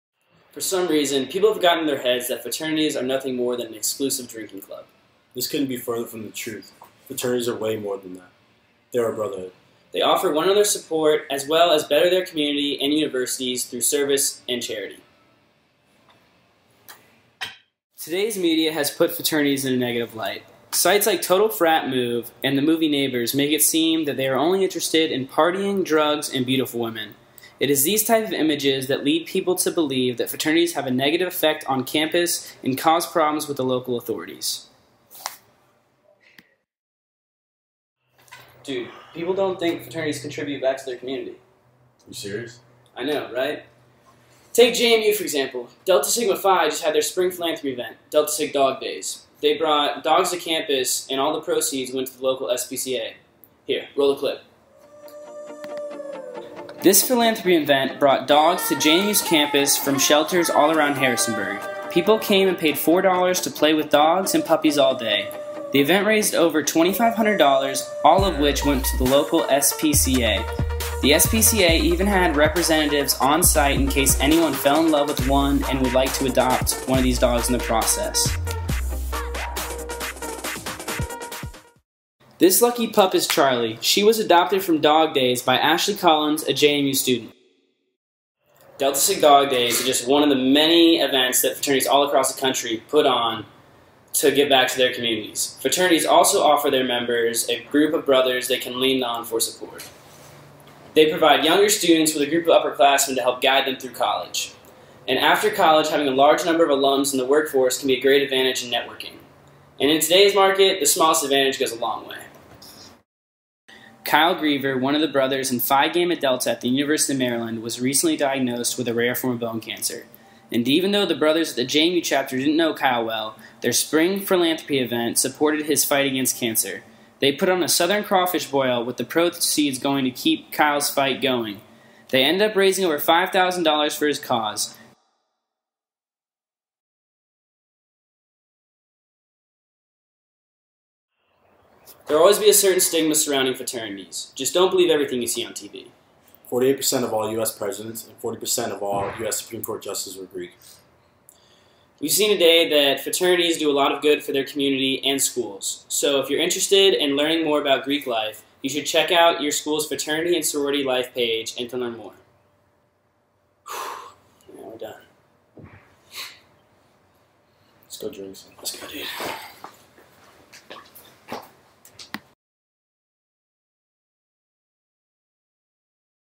Alex frat video
Alex Hudgins. this is a satirical video made to persuade people to see Fraternities as more than a drinking club.